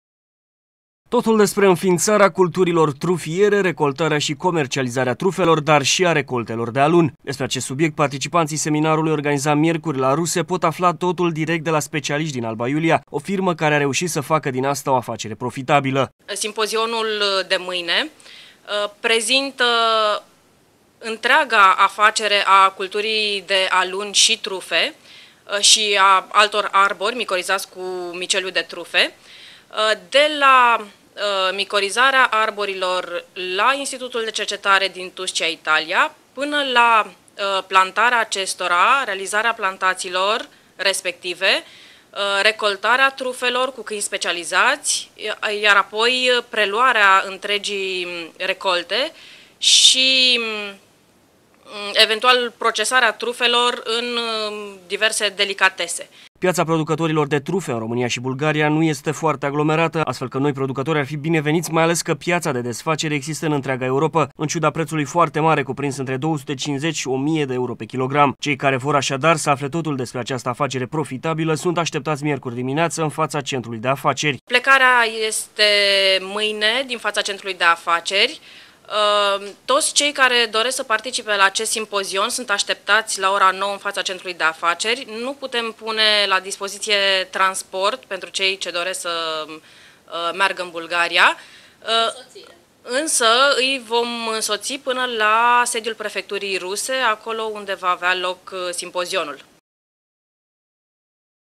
Trufele, o alternativă pentru agricultori
Producatorii agricoli români şi bulgari, dar si  specialistii din domeniul administratiei publice pot alfa mâine, la Ruse, în cadrul unui simpozion, cum pot pune pe picioare o afacere cu trufe, de la a la z. Evenimentul este organizat de Centrul de Afaceri si ii va avea ca invitati speciali pe reprezentantii unei firme din Alba Iulia, specialisti in acest domeniu.